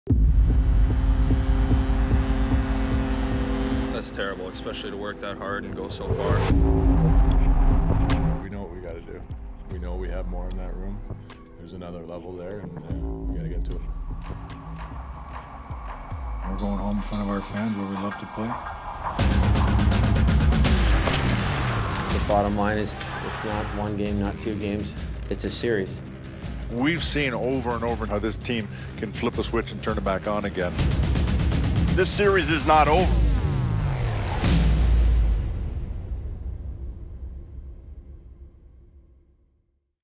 LA Kings vs. Vegas - Game 3
The LA Kings return to STAPLES Center for Game 3 of Round 1 of the 2018 Stanley Cup Playoffs. Kings Live starts at 7pm on Fox Sports West